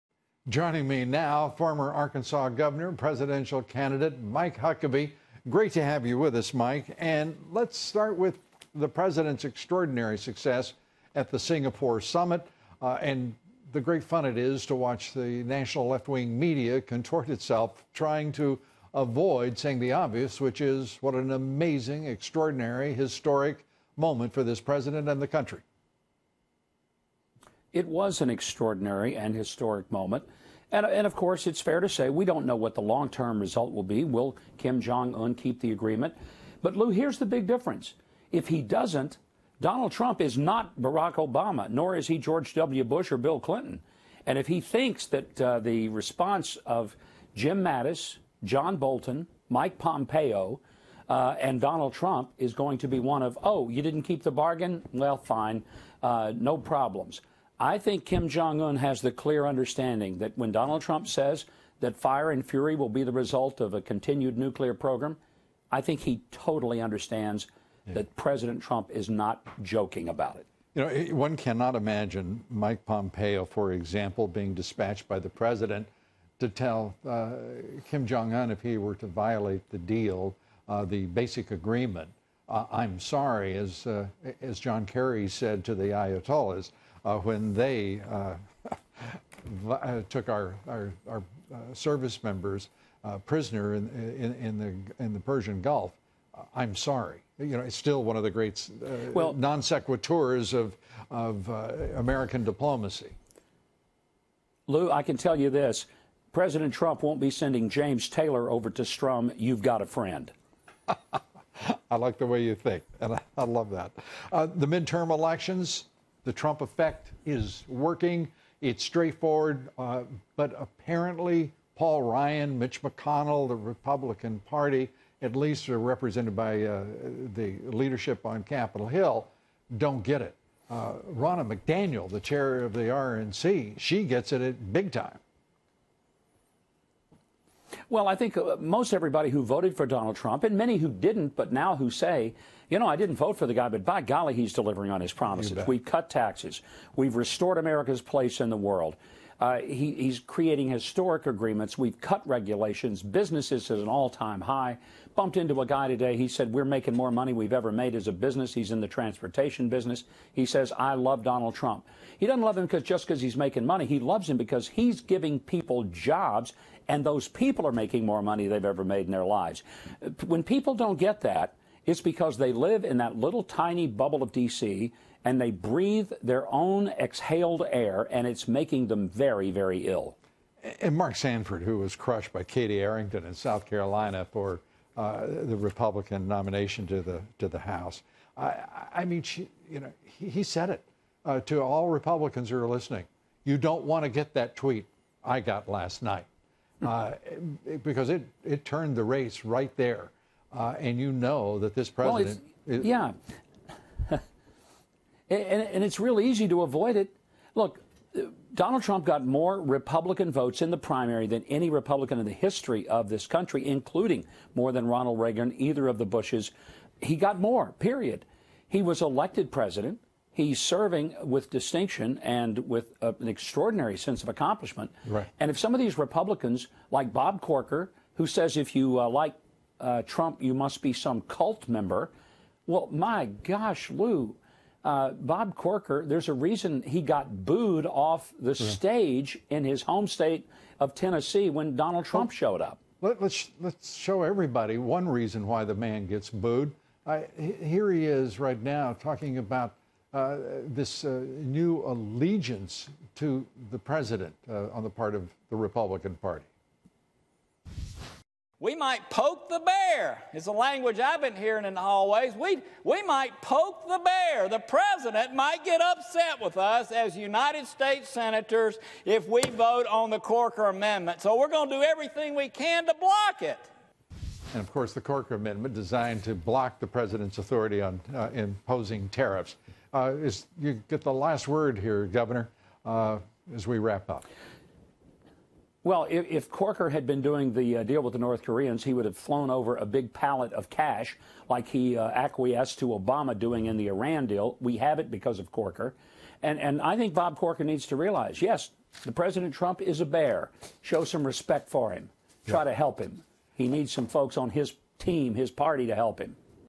Senator Bob Corker should show Trump some respect: Mike Huckabee
Former governor Mike Huckabee (R-Ark.) discusses President Trump’s meeting with Kim Jong Un and why Senator Bob Corker (R-Tenn.) should start respecting the president.